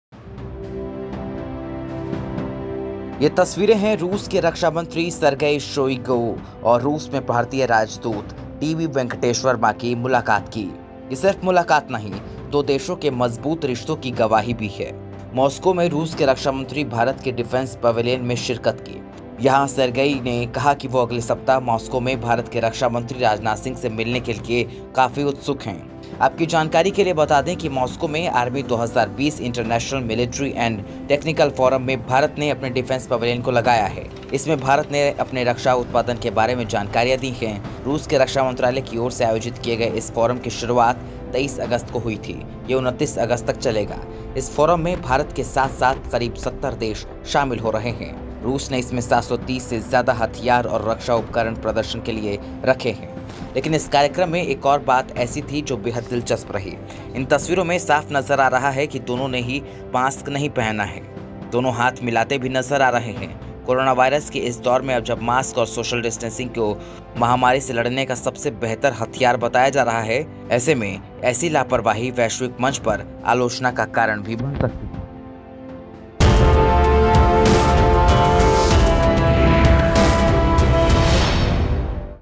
भारत के रक्षा मंत्री RAJNATH SINGH से मिलने को बेताब है रूस!
रूसी रक्षा मंत्री सर्गेई शोइगू शुक्रवार को मॉस्को के बाहर कुबिंका में आयोजित हो रहे एक रक्षा फोरम में भारत के डिफेंस पैविलियन में शिरकत की। यहां रूस में भारतीय राजदूत डीबी वेंकटेश वर्मा ने उनका स्वागत किया। शोइगू ने कहा कि वह अगले सप्ताह मॉस्को में भारत के रक्षा मंत्री राजनाथ सिंह से मिलने के लिए उत्सुक हैं।